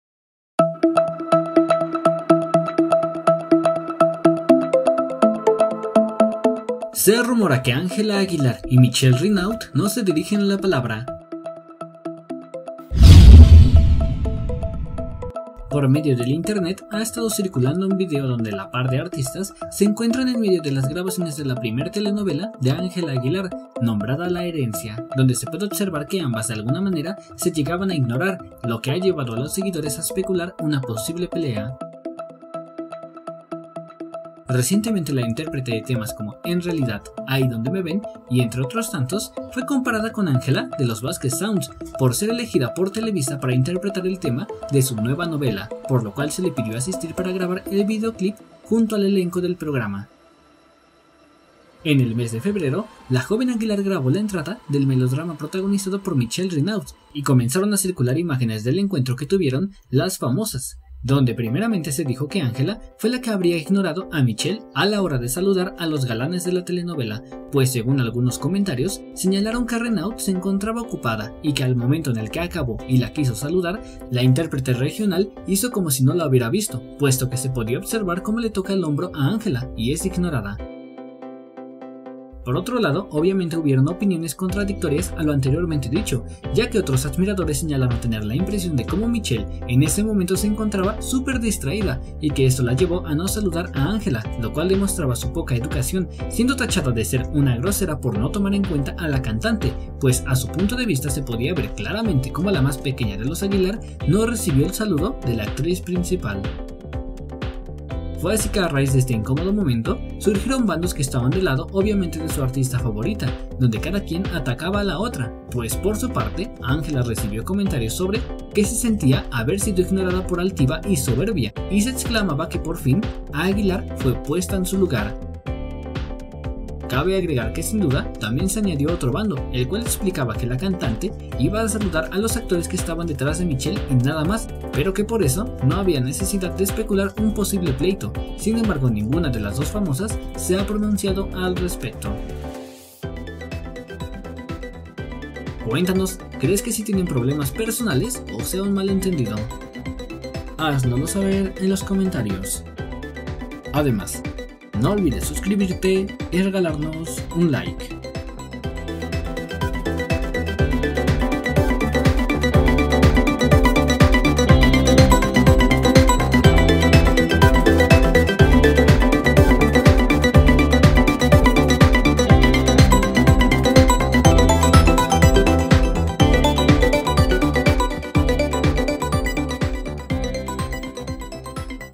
¡TREMENDO DESPLATE!… Ángela Aguilar IGNORA a Michelle Renaud.
Se rumora que Angela Aguilar y Michelle Renaud no se dirigen la palabra